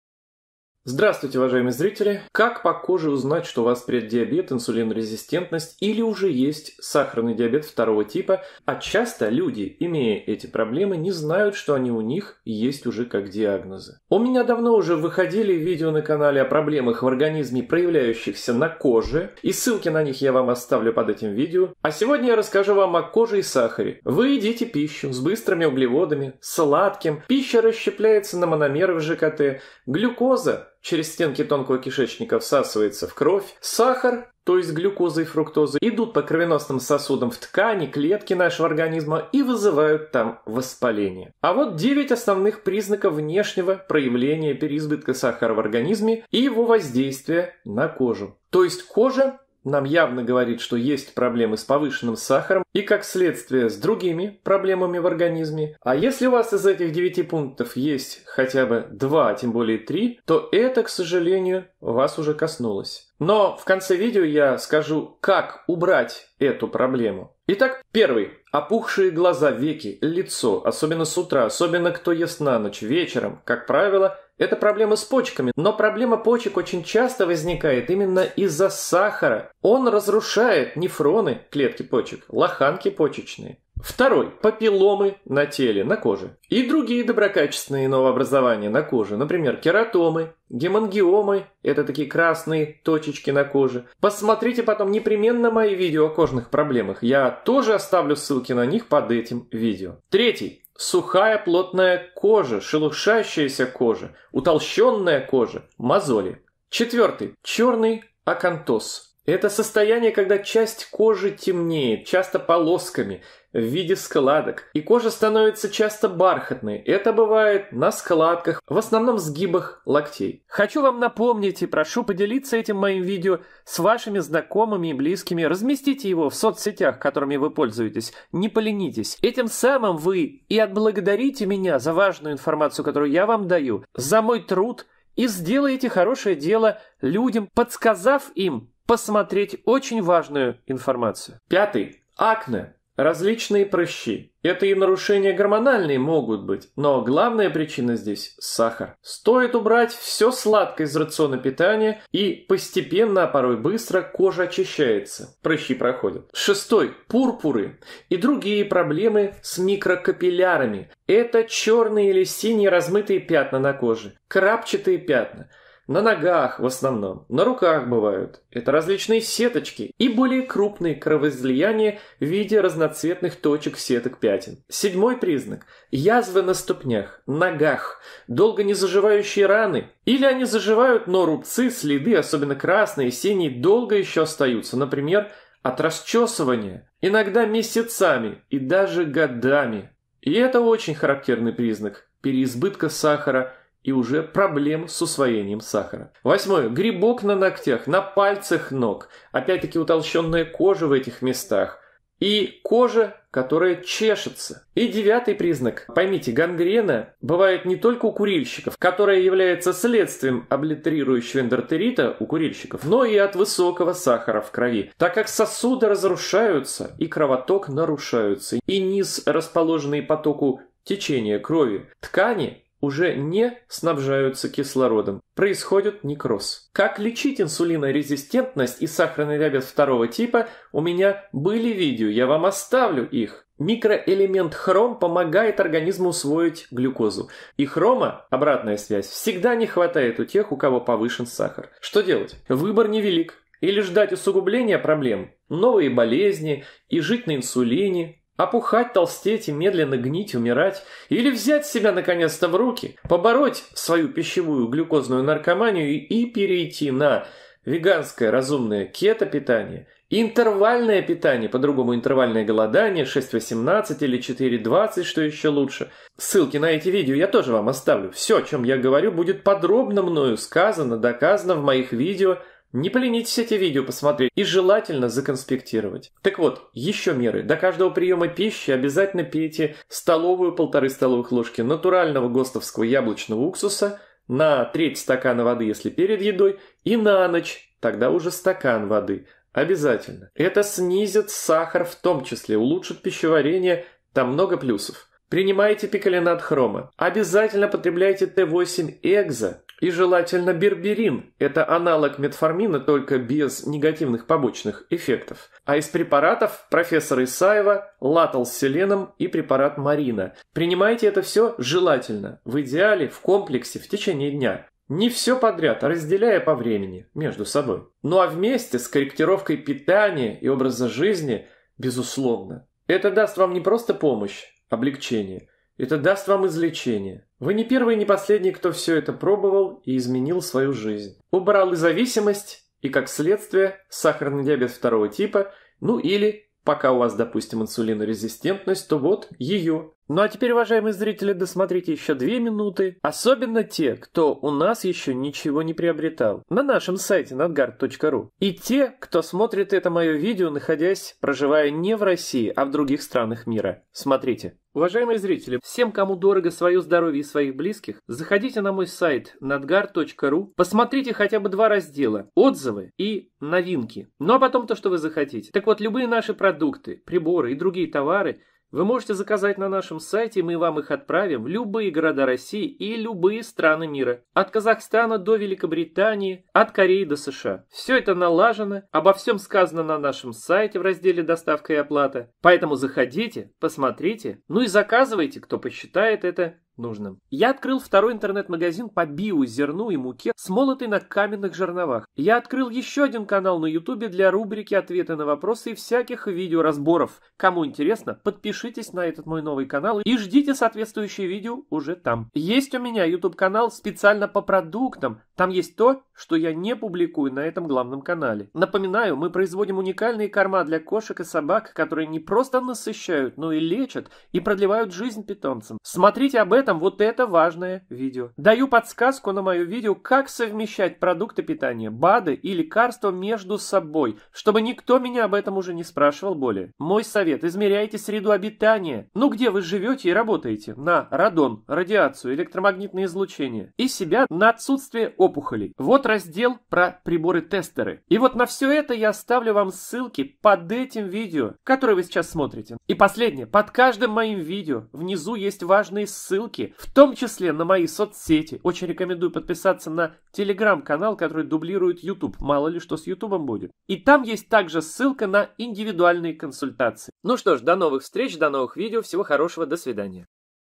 Как Ваша КОЖА говорит, что у Вас повышен сахар, или УЖЕ есть сахарный диабет 2 типа! Вот 9 ЗНАКОВ!
Какая связь кожных проблем с сахаром? Как по коже узнать, что у Вас преддиабет, инсулинорезистентность, или уже есть сахарный диабет 2 типа, но пока не явный для Вас? Часто, люди, имея эти проблемы, даже не догадываются о их существовании, т к привыкли к постоянной утомляемости, слабости, упадку сил! Ниже даны очень важные ссылки:

Сайт по БИО зерну, бобовым и пр.

👁️ Навигация (тайм-коды):
0:00 Вступление. О чем будет данно видео?
1:08 9 признаков внешнего проявления переизбытка сахара
5:43 Как купить и получить то, что нужно?